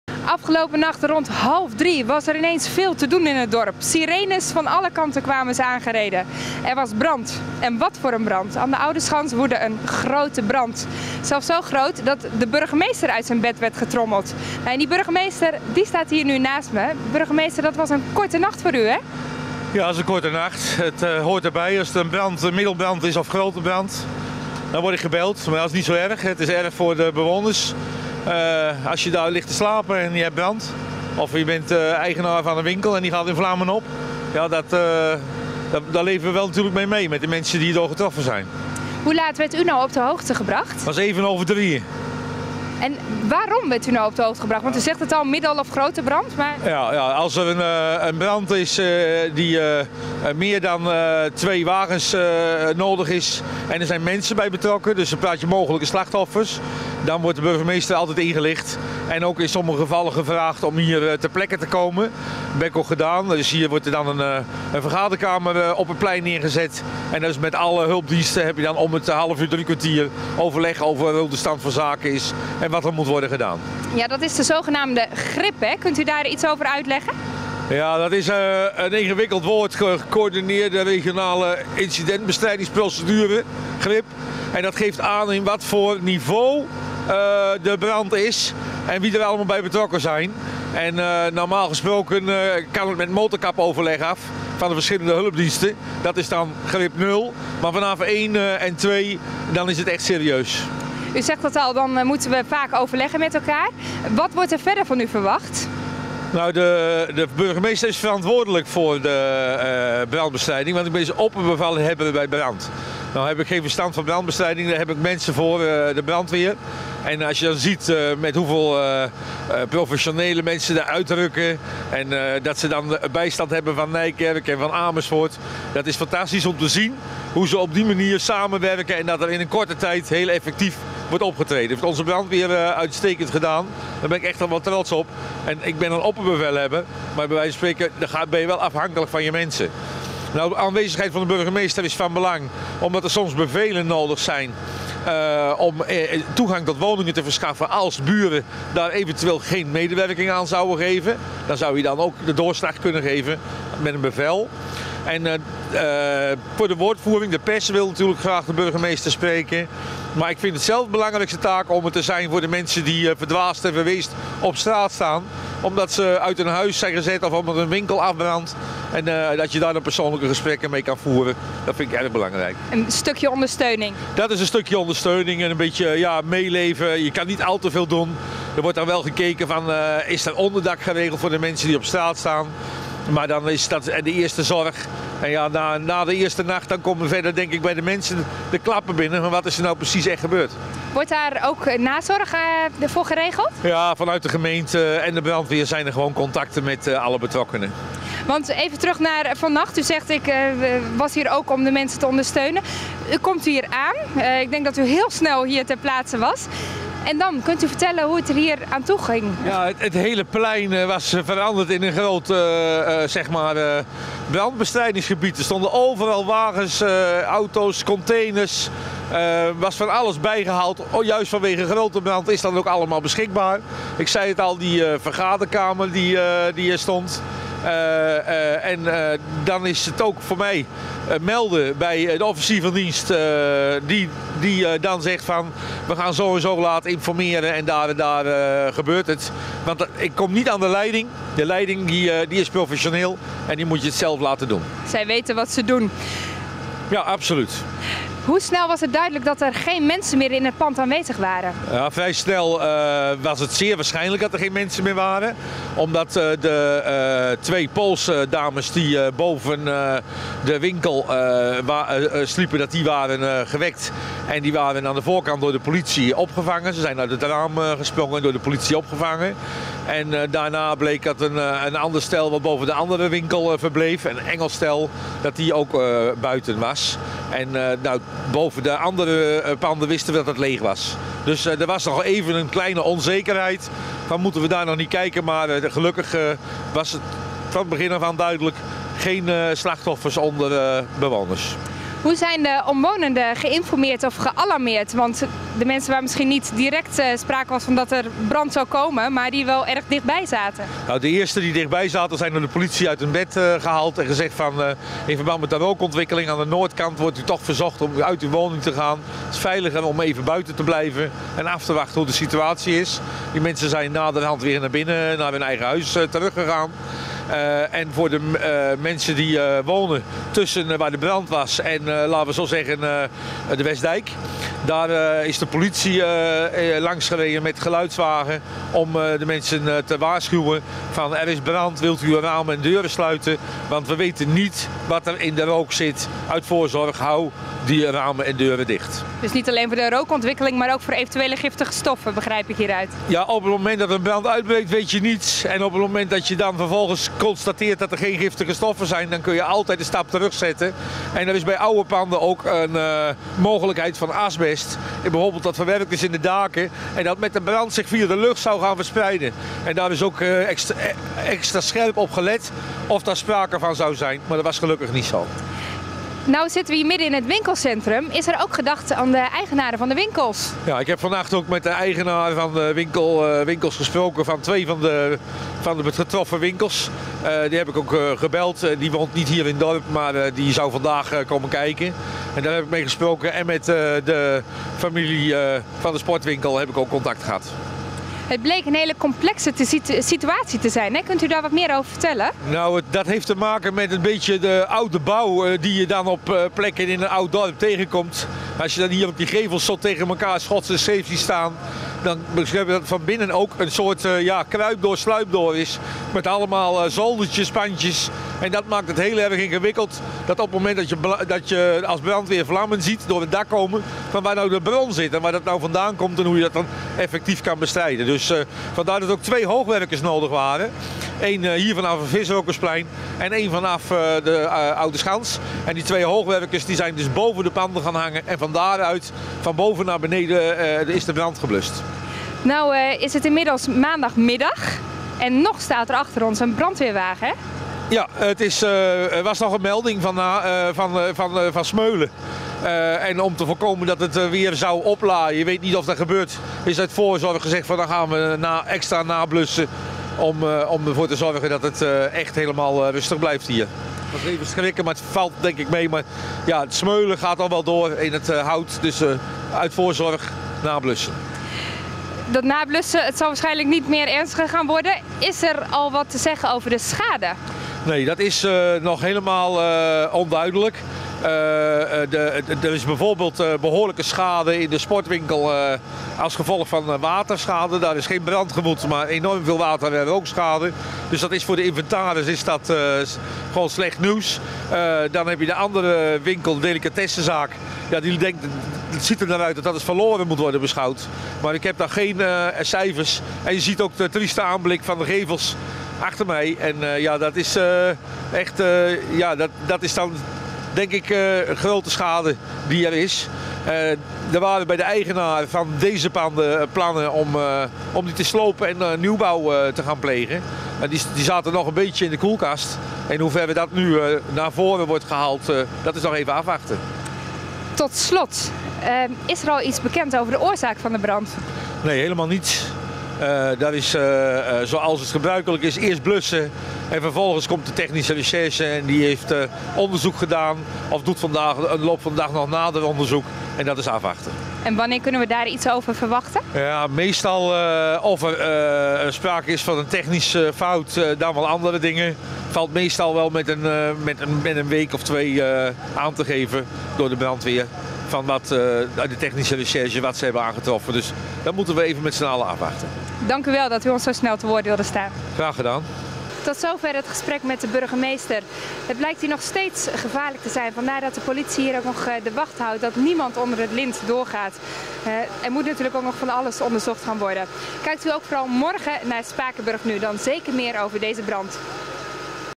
Brand op het Spui
Een gesprek met burgemeester van de Groep over de brand en het calamiteitenplan wat hierbij komt kijken.

Opgenomen op 9 april 2012
LOS ( Lokale Omroep Spakenburg )